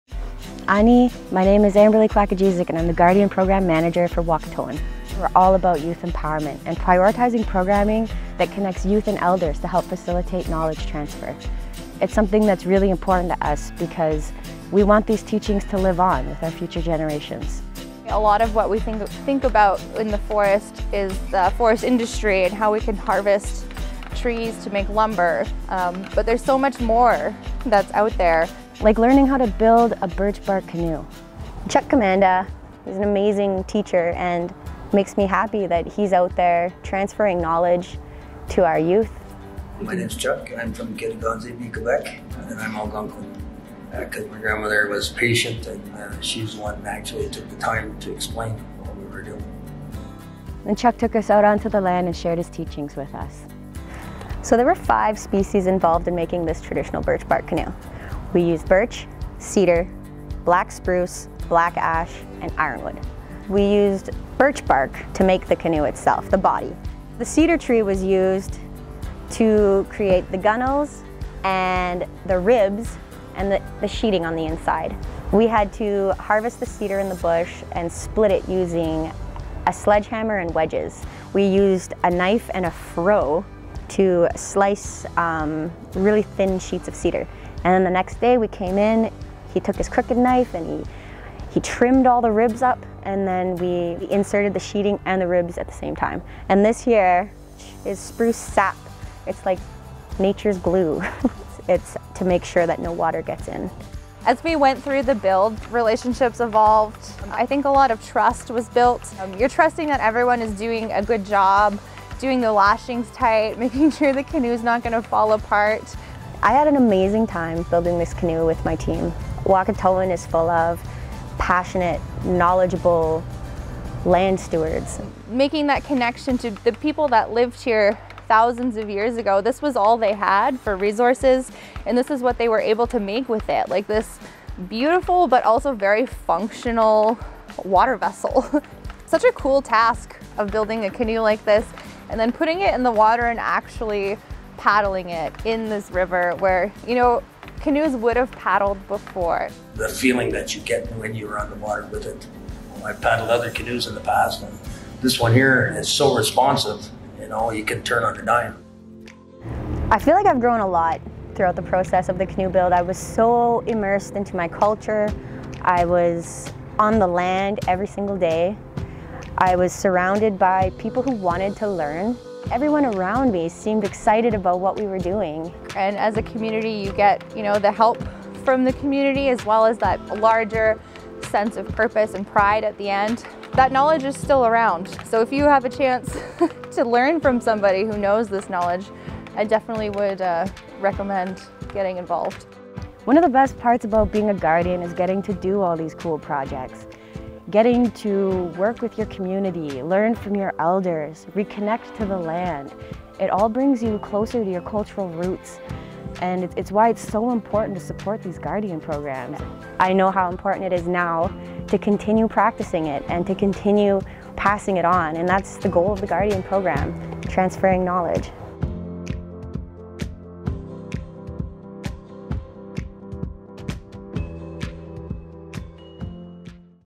Canoes, Community & Connection
Building a birch canoe by hand takes more than just birch bark: it takes community, generations of knowledge -- and a little bit of spruce sap. Land Needs Guardians Storytellers and Wahkohtowin Guardians Amberly Quakegesic & Isabelle Allen share how the experience brought them closer to their culture and reconnected them to the land, Elders, and their community.